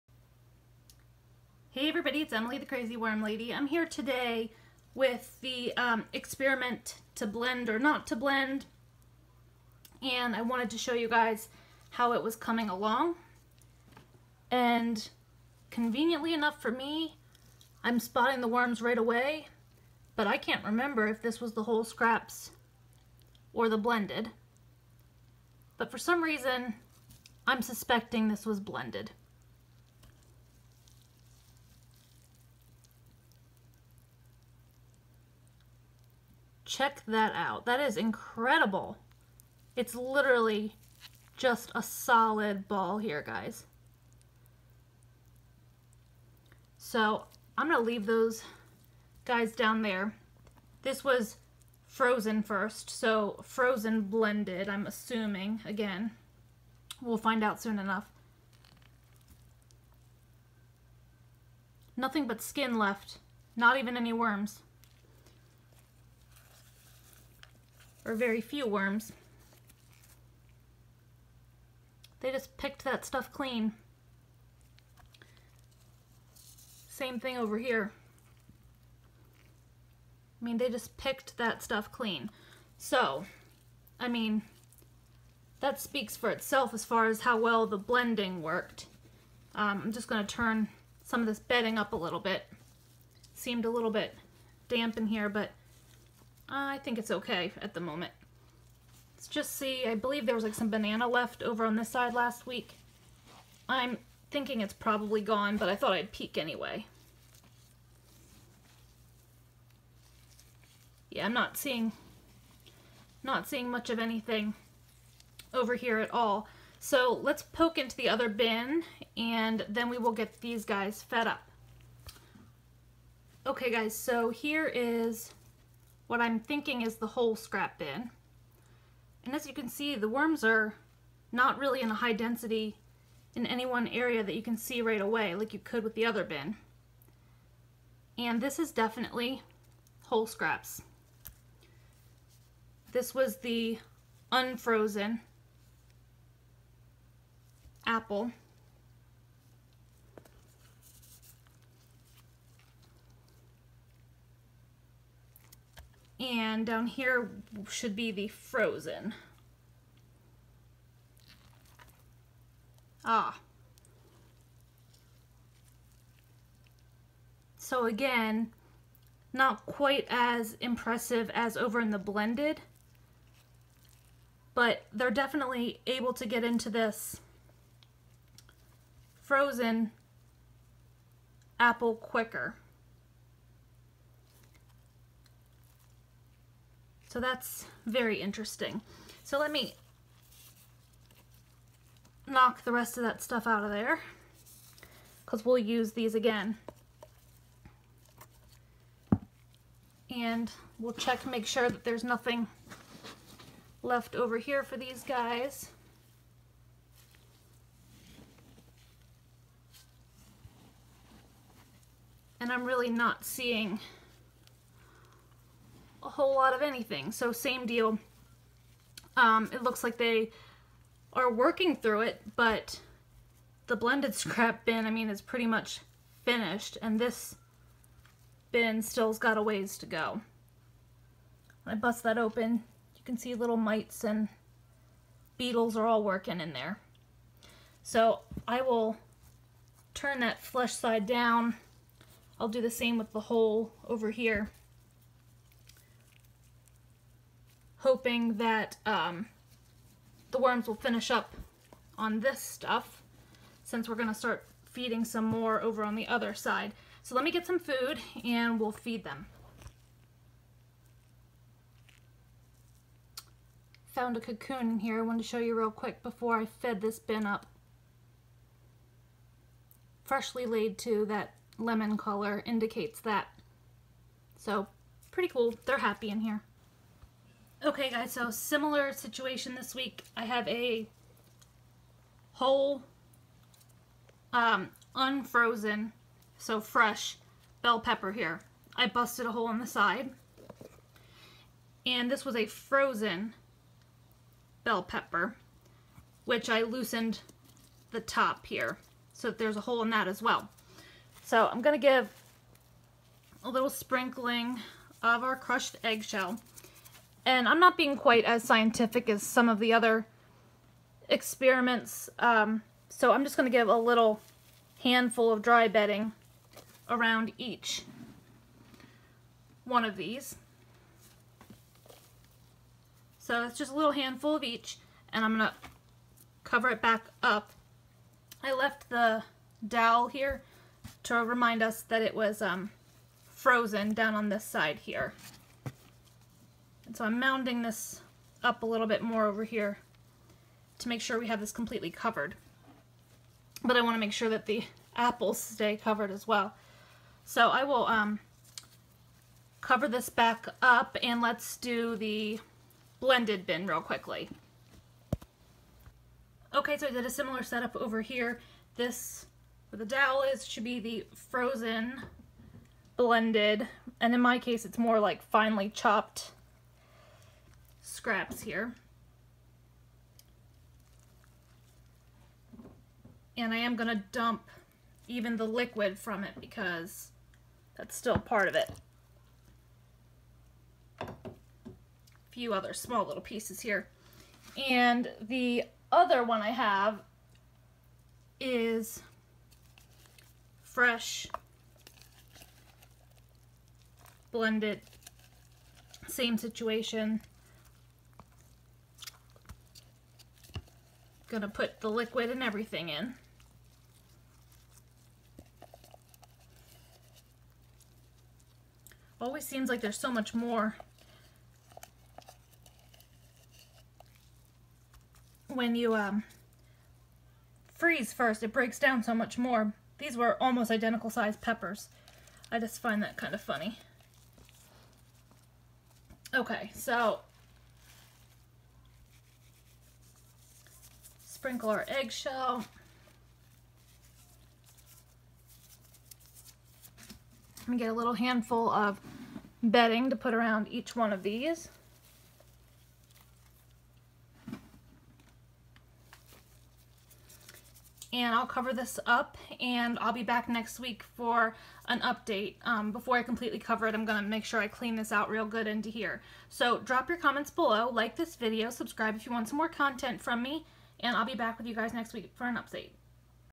To Blend or Not to Blend Update
Experiment: to Blend or Not to Blend Update!

Interesting results.  Blended food is almost entirely gone.  Frozen blended is 100% gone.  The whole scraps are barely breaking down at this point.

New feeding today following the same methodology.  Including the frozen aspect.

Emily (The Crazy Worm Lady)